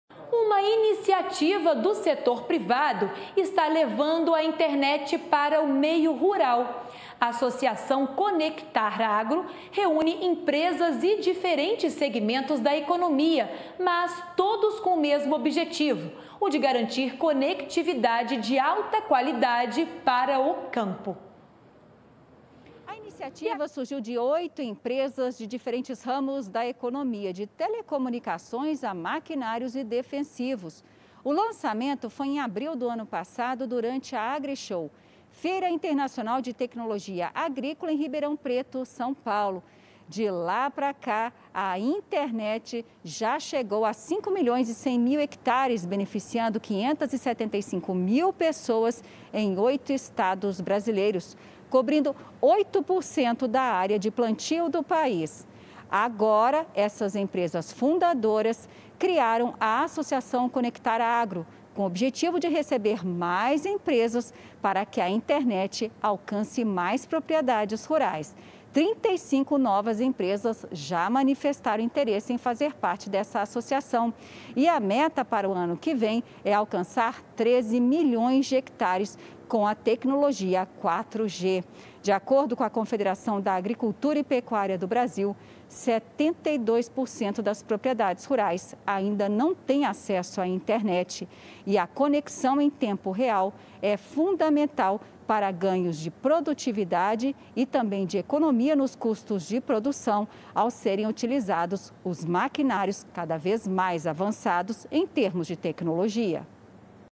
Iniciativa do setor privado leva internet para o meio rural | AgroMais
A Associação ConectarAgro reúne empresas e diferentes segmentos da economia com o objetivo de garantir conectividade de alta qualidade para o campo.